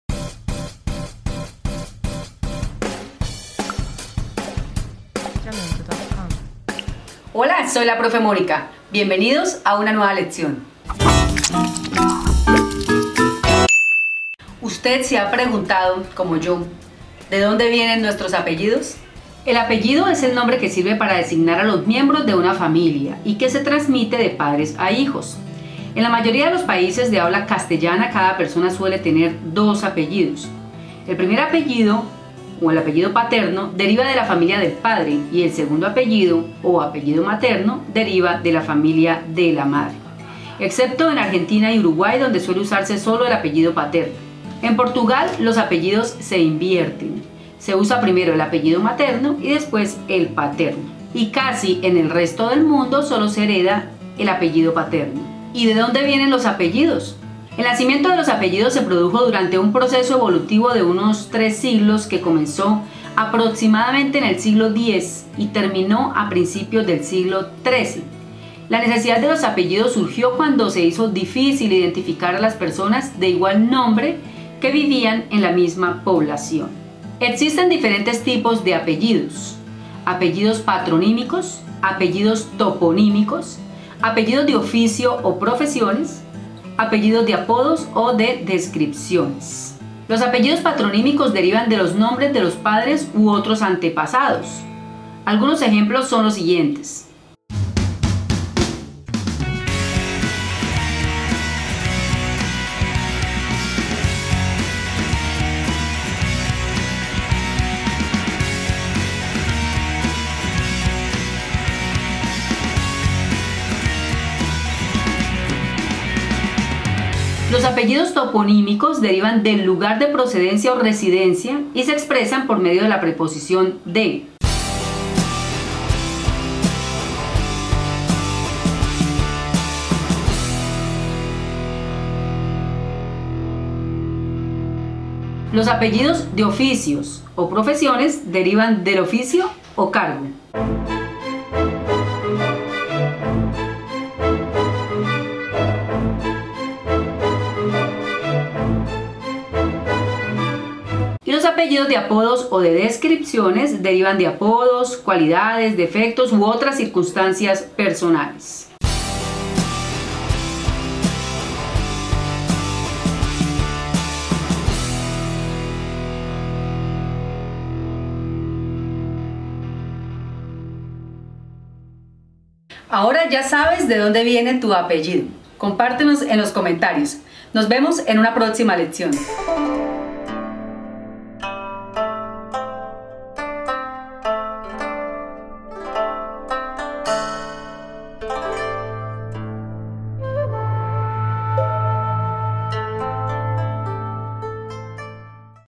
EL CURIOSO origen de los APELLIDOS
¡Comenta tu apellido y buscamos su origen! 😍 Esta es una de las preguntas que me hago cada día. ¿De dónde viene mi apellido? Y aunque no tengo la certeza de esta historia, indagué acerca de este tema.